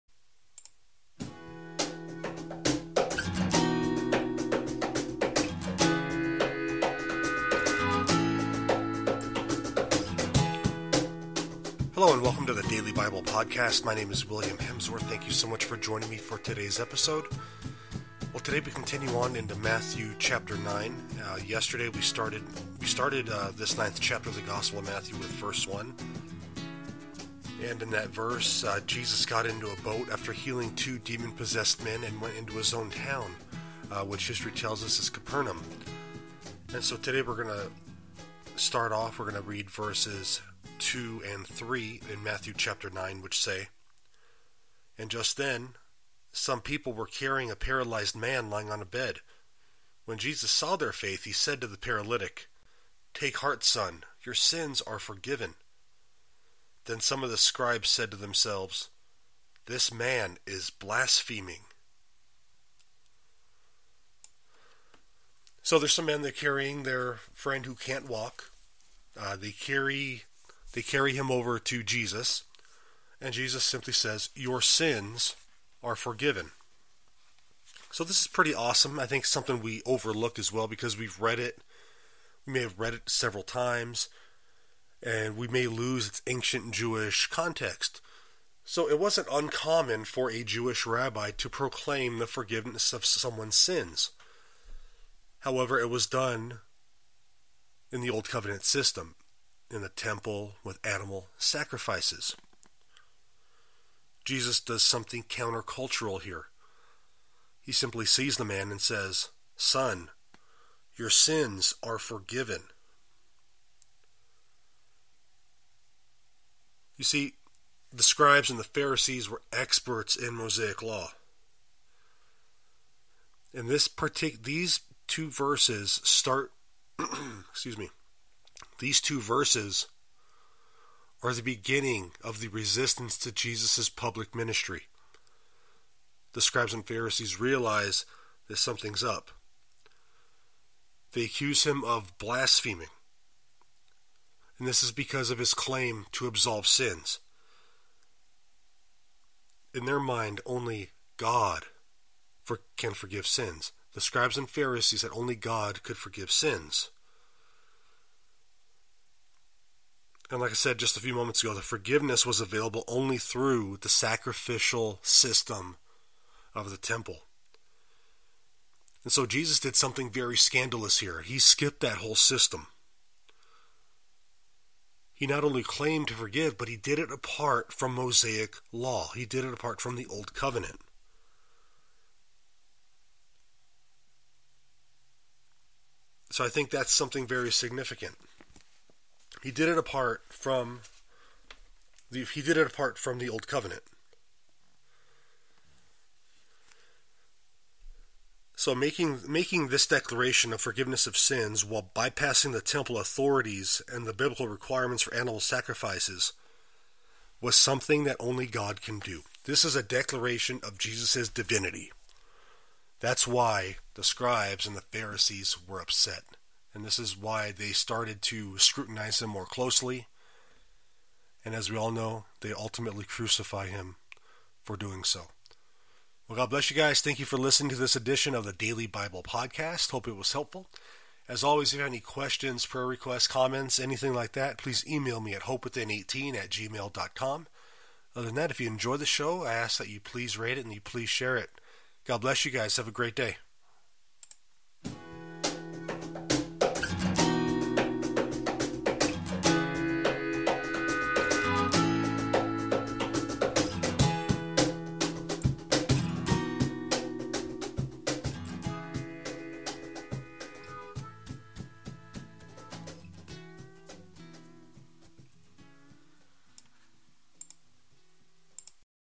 Matthew 9:2-3
People carry a paralyzed man to Jesus and Jesus forgives his sins.  The scribes and Pharisees accuse him of blasphemy.